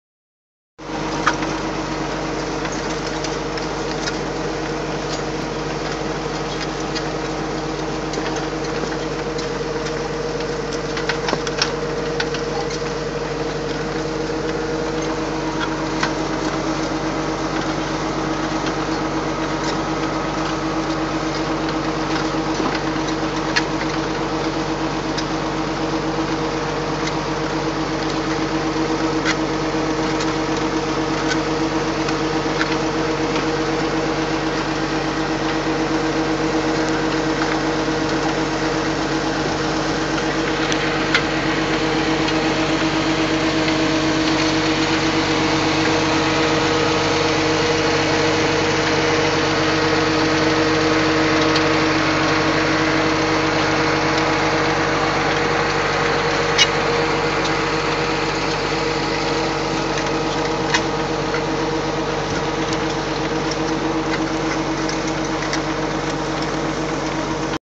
Plastic layer in action
June 15, 2014  Plastic laying machine putting down an 800 foot row.